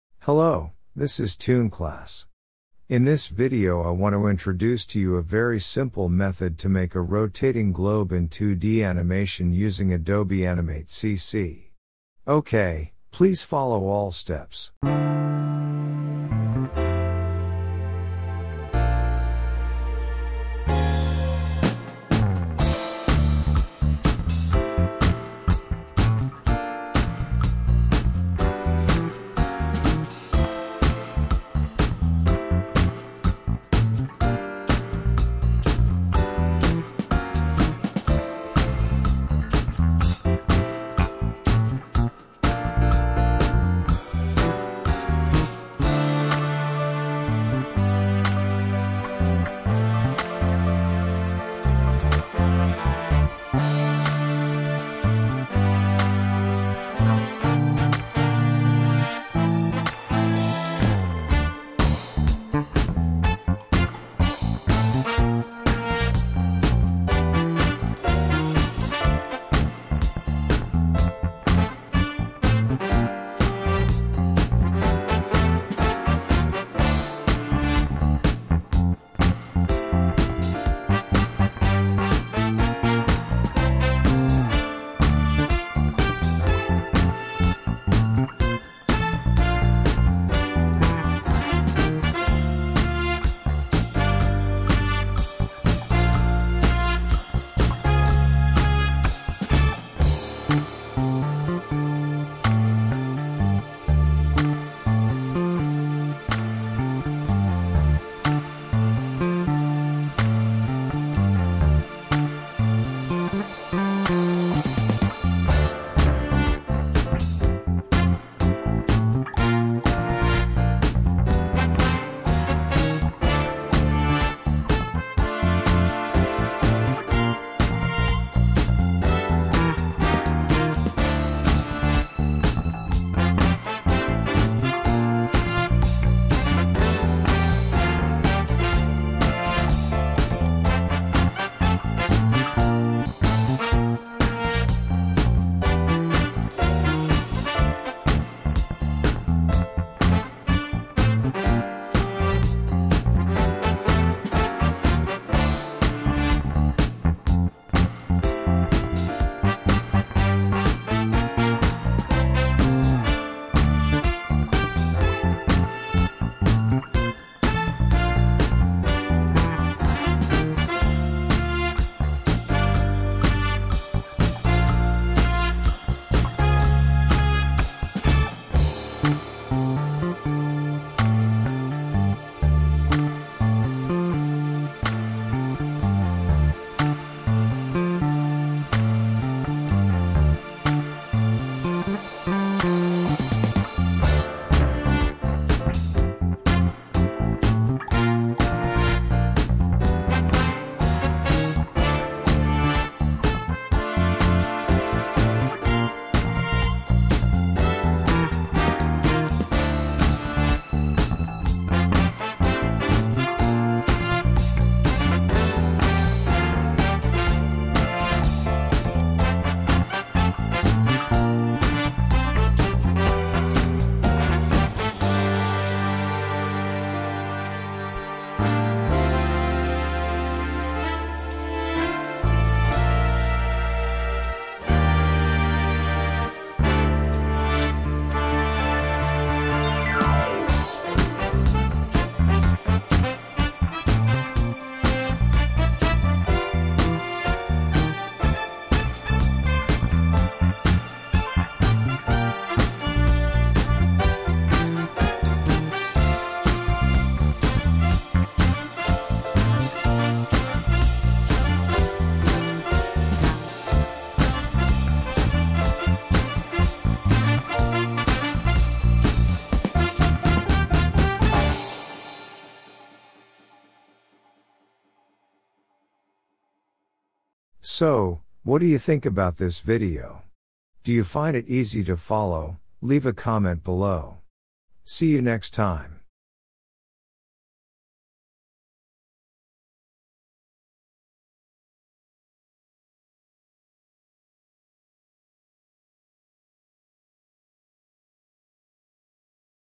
Adobe Animate #10: Very Simple Method to Make Rotating Globe
This video shows you a very simple method to make a rotating globe or world map animation using adobe animate cc. Hope you like this. Thank you!